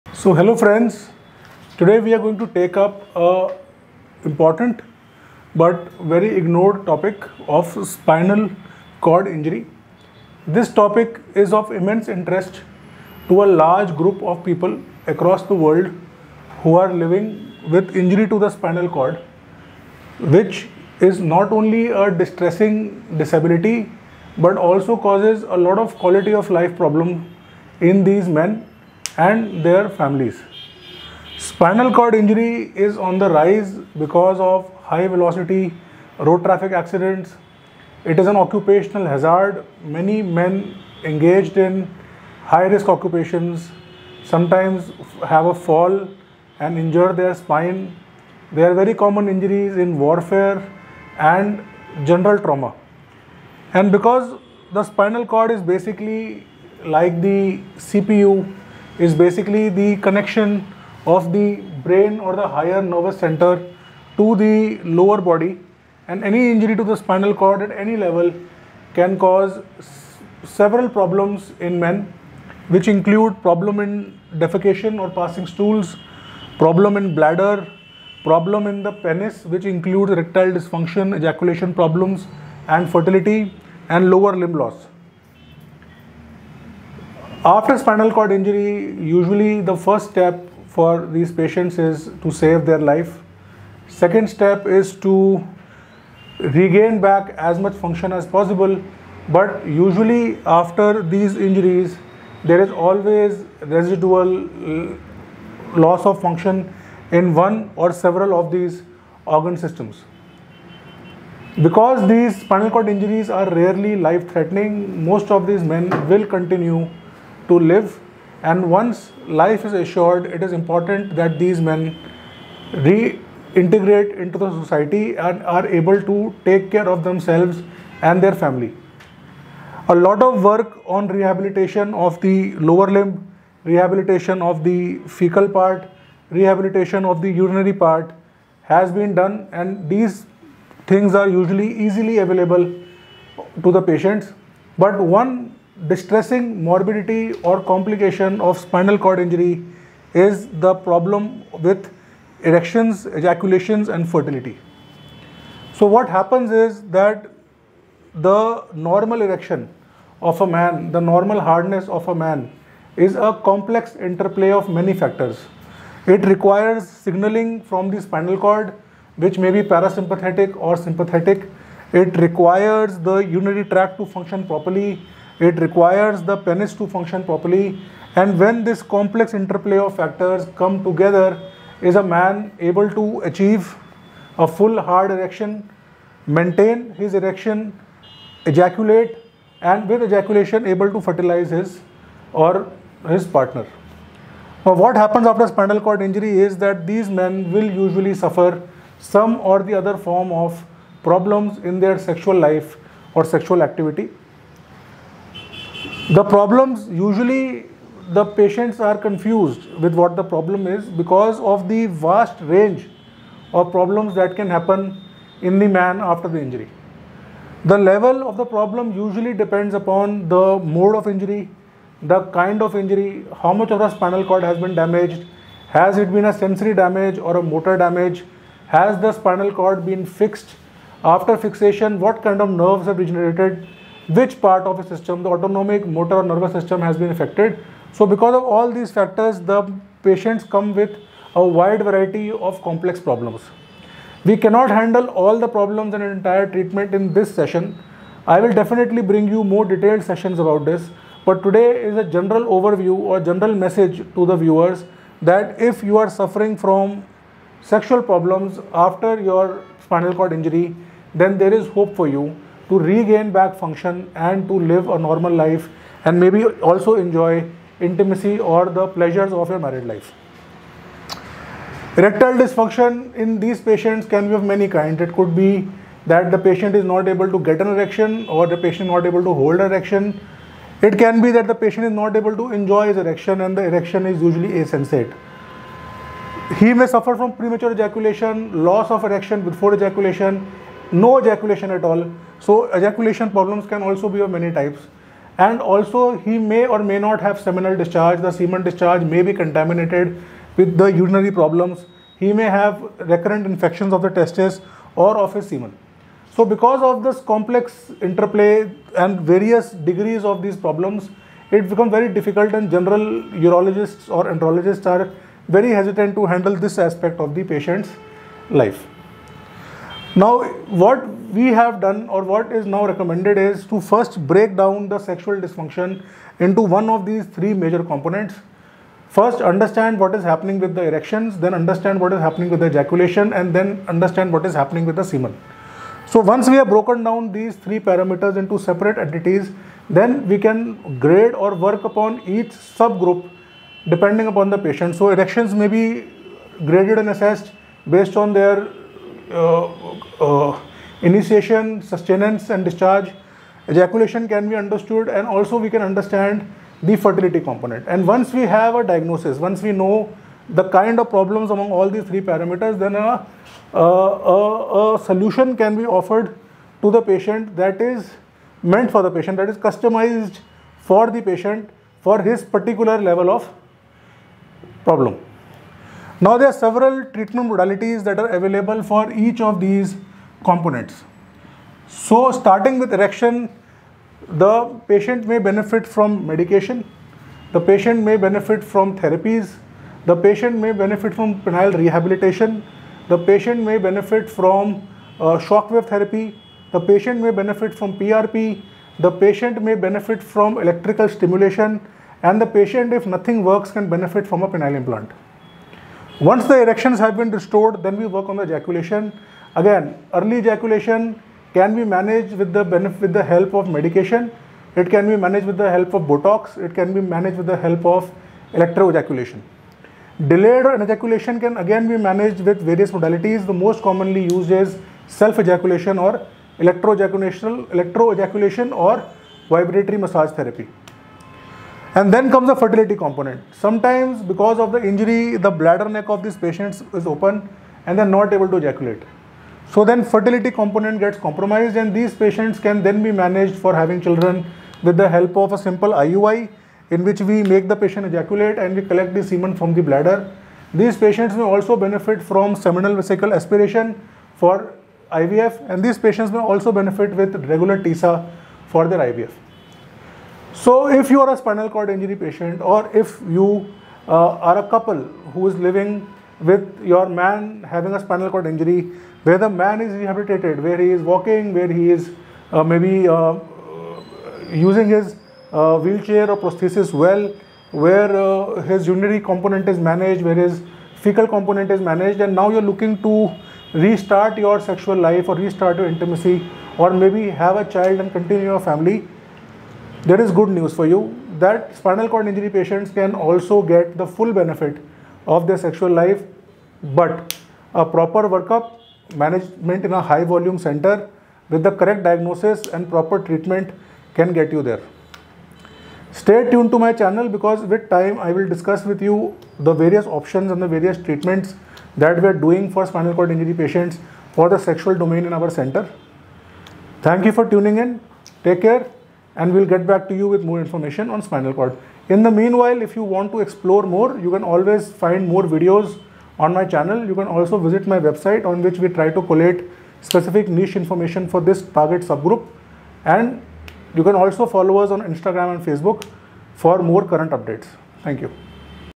Spinal Cord Injury Recovery - Sex and Fertility
Spinal cord Injury is morbid. Disability after spinal cord Injury is immense. Of all aspects sexual function and Fertility in spinal cord injury is most difficult to recover

In this video learn about resuming sex after spinal cord Injury Recovery.

Sex and Spinal Cord Injury | SCI Recovery | Erections and Spinal Cord Injury | Premature Ejaculation after SCI | Spinal Cord Injury and fertility

Note : This is a medical Information Video for Patients.

Know More
Address 27B/4, New Rohtak Road, Karol Bagh New Delhi 5